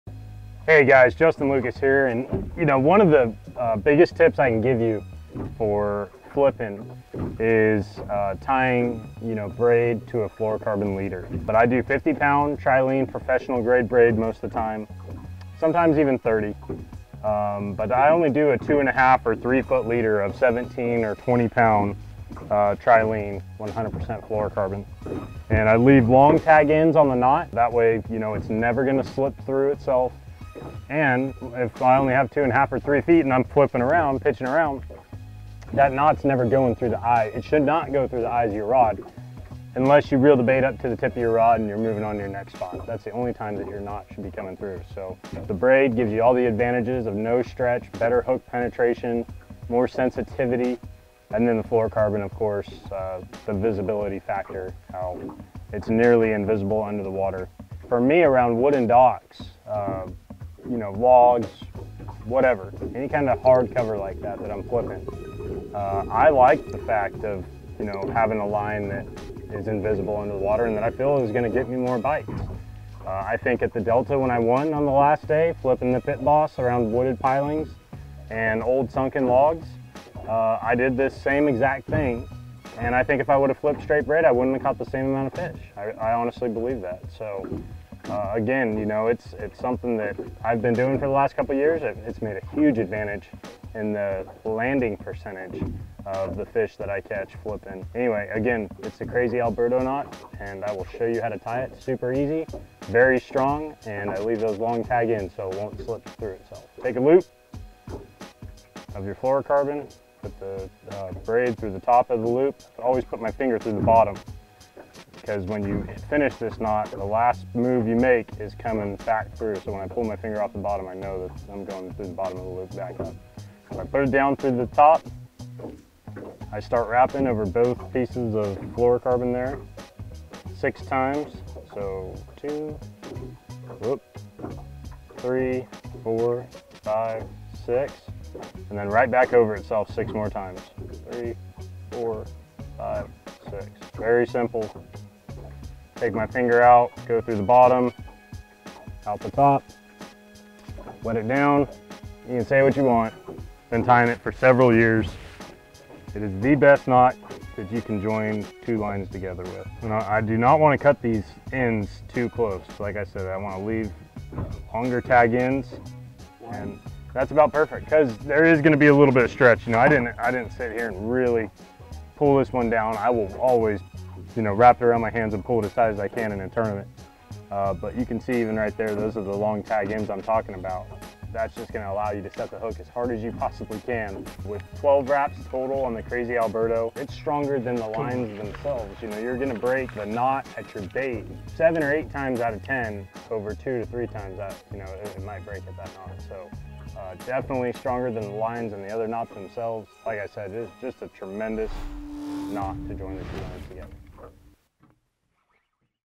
Why You Should Add a Fluorocarbon Leader to Your Flipping Setup
Most experienced anglers add a fluorocarbon leader to their braid setup on spinning gear This adds stealth, as fluorocarbon is a lot less visible than braid, plus a little stretch forgiveness to an otherwise stretch-free braid setup. A lot less common is doing this with casting gear, and specifically with heavy flipping and pitching setups.

Professional angler Justin Lucas believes a few feet of heavy low visibility fluorocarbon increases the number of bites he gets, especially around hard cover such as wood and docks. These areas provide less concealment compared to grass, which effectively hides braid. His knot of choice is the Alberto aka Modified Albright. As the video shows, tying it is simple and most importantly, tough and fail-proof according to Lucas.